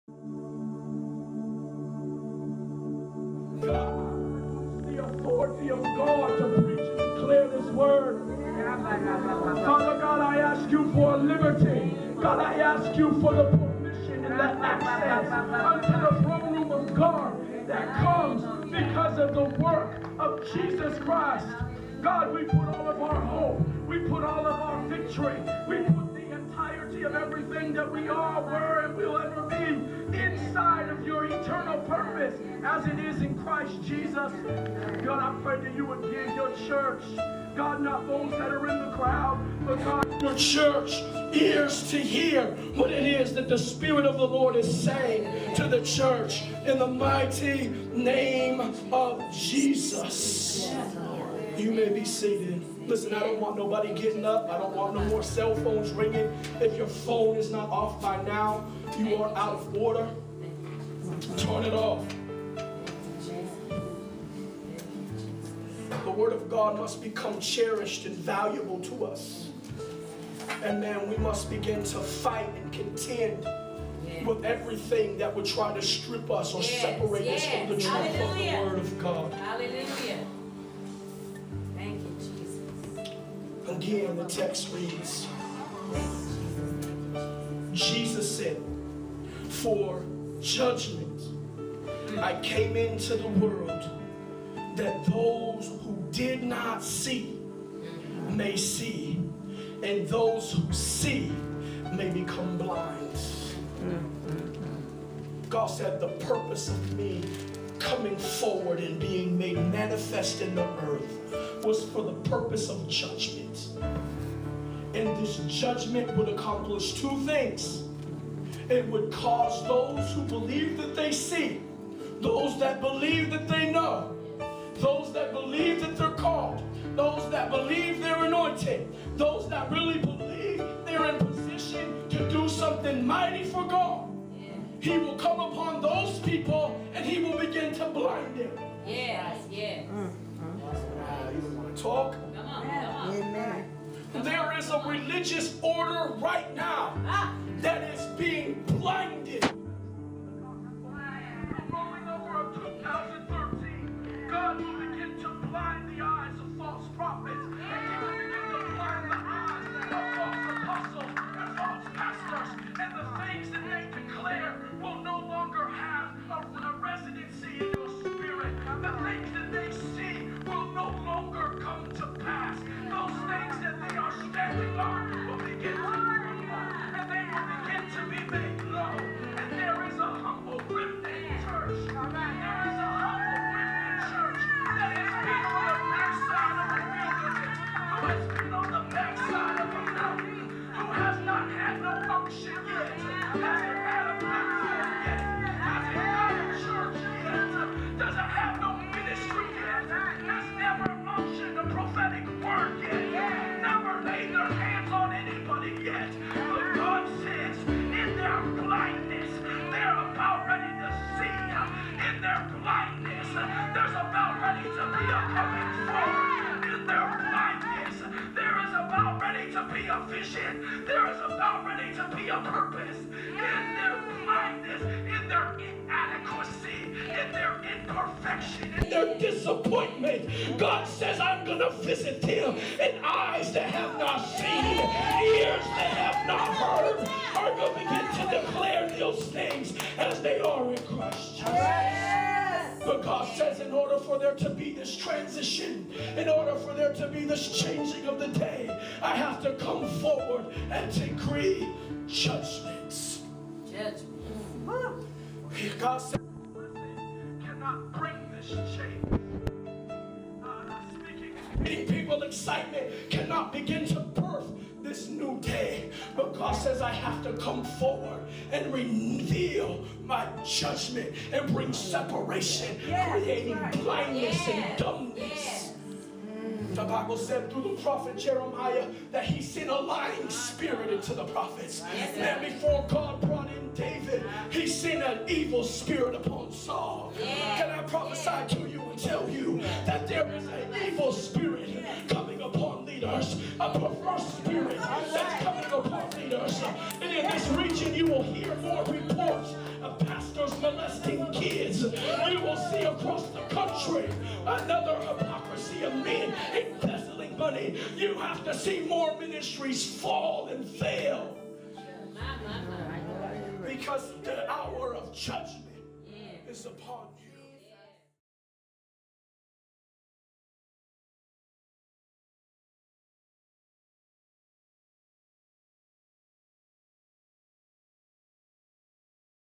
Hour Of Judgement- (Prophetic Word For 2013)
This was preached right after the first of the year 2013, and we are sad to report ministries have already fallen, and in the news it was already reported more children have been touched. Dear believer we are living in peril times, Be Alret,Watch and Pray.
Counting myself as nothing
jeromy